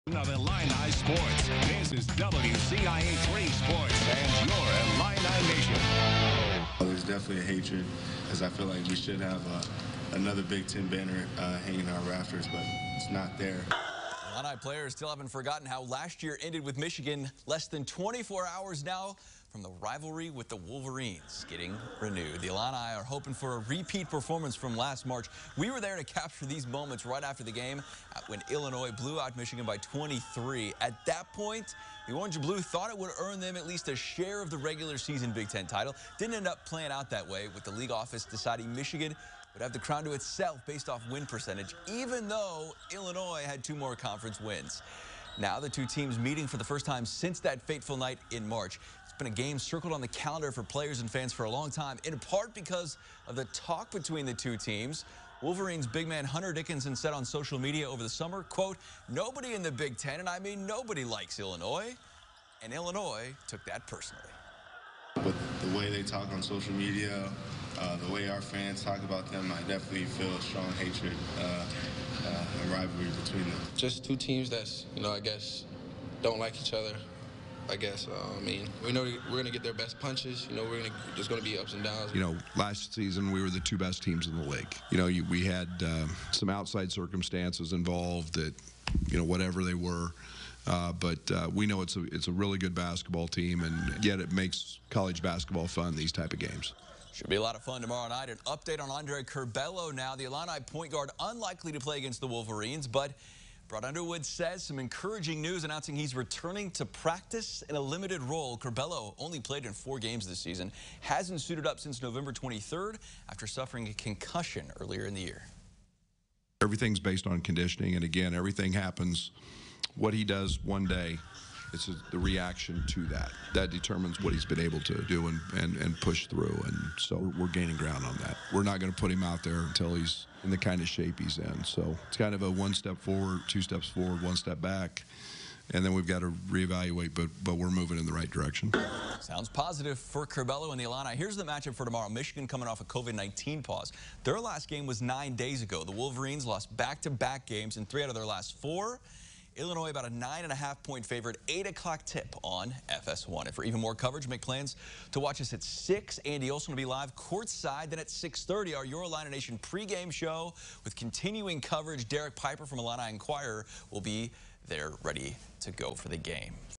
Illinois ready to renew rivalry with Michigan: "Just two teams that don't like each other"
CHAMPAIGN (WCIA) -- There is no love lost between Illinois and Michigan, something the Illini are not hiding heading into Friday night's rivalry game at State Farm Center. Not after what happened last year.